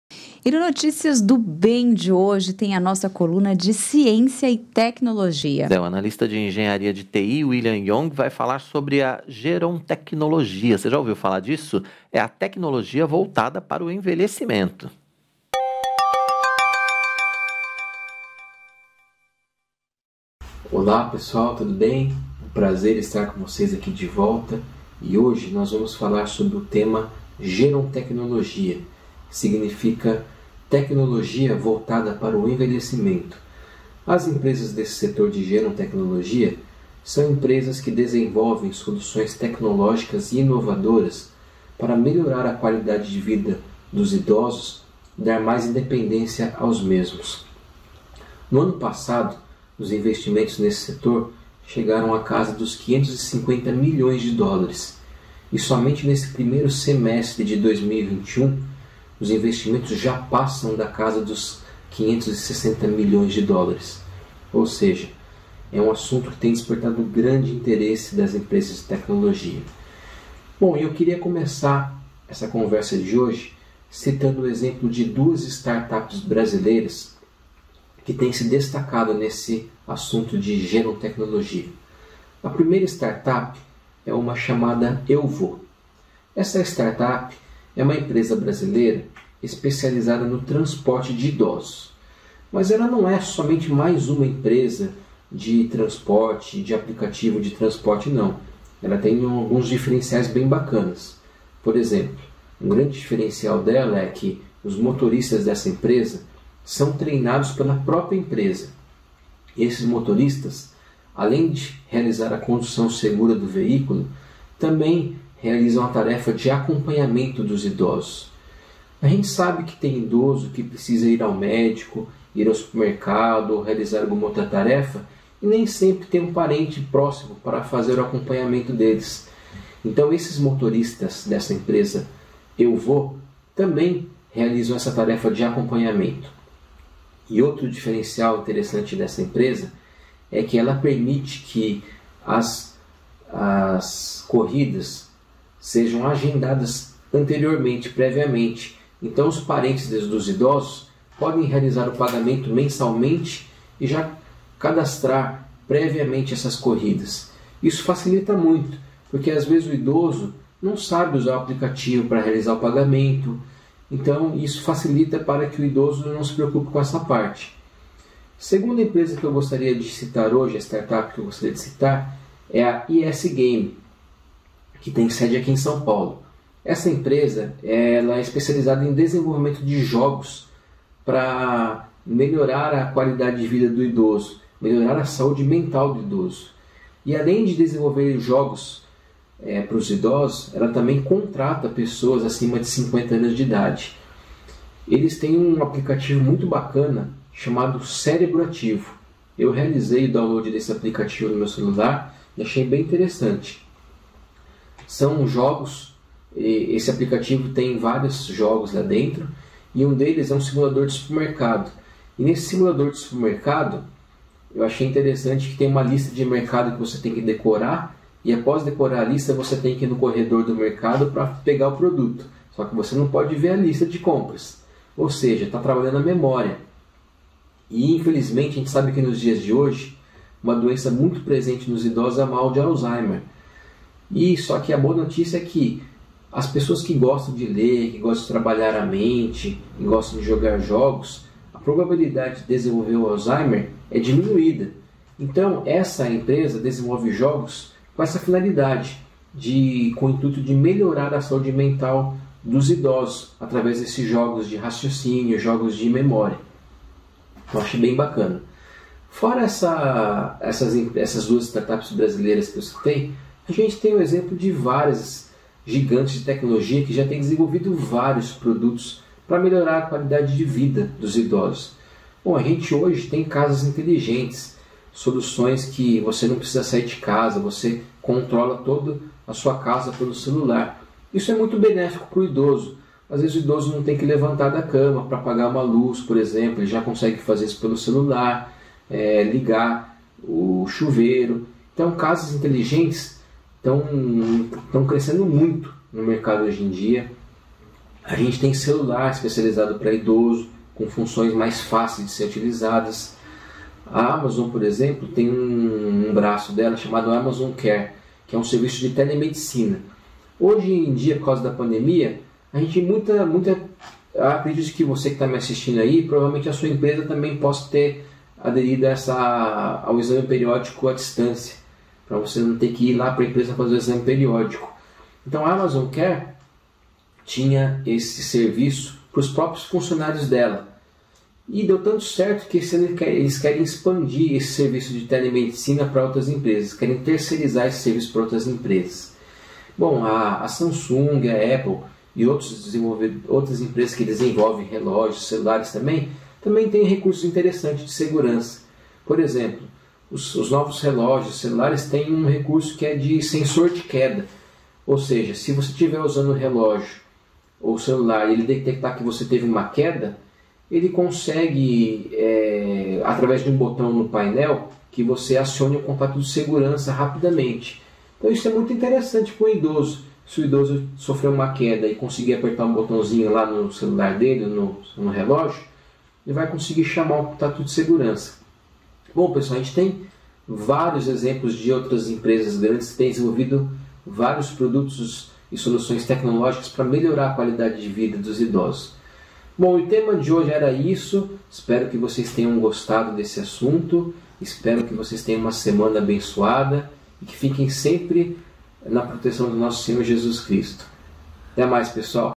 COLUNA CIÊNCIA E TECNOLOGIA - ENVELHECIMENTO
Se você nunca ouviu falar em gerontecnologia, nosso colunista William Yong traz todas as informações sobre esse trabalho voltado aos idosos que cresce cada vez mais.